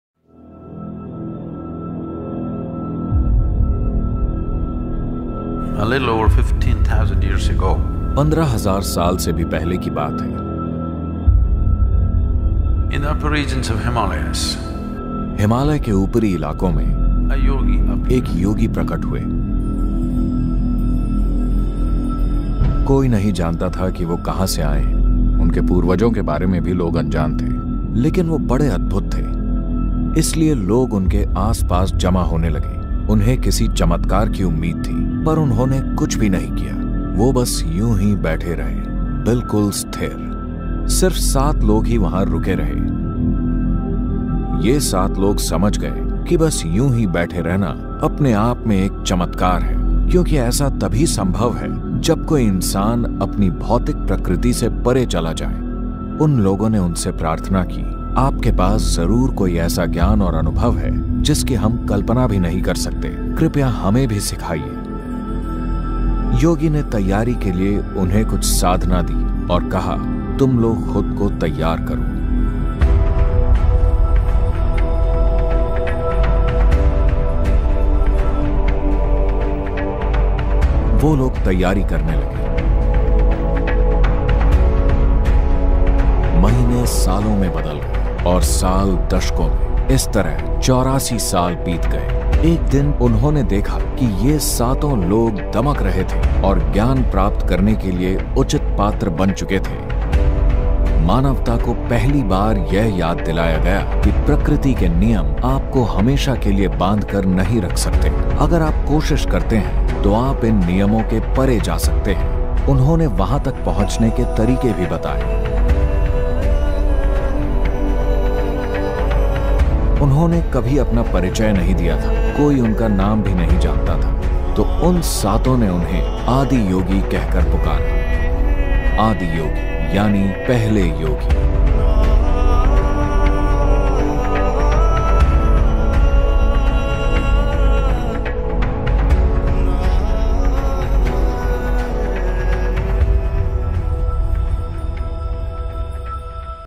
आदियोगी: योग का रहस्य और शिव का अनावरण | Adiyogi | Sadhguru Sadupdesh
Adiyogi: The Secret of Yoga and the Unveiling of Shiva

📆 सीमित समय!

📆 Limited time!

क्या आपने कभी सोचा है कि योग कैसे शुरू हुआ? भगवान शिव, जिन्हें योग का आदिगुरु माना जाता है, उन्होंने कैसे इस अद्भुत विज्ञान को दुनिया को दिया? इस वीडियो में, सद्गुरु हमें आदियोगी, भगवान शिव के विशाल रूप की यात्रा पर ले जाएंगे और योग के रहस्यों को उजागर करेंगे।

Have you ever wondered how yoga began? How did Lord Shiva, who is considered the Adiguru of yoga, give this wonderful science to the world? In this video, Sadhguru will take us on a journey of Adiyogi, the gigantic form of Lord Shiva and reveal the secrets of yoga.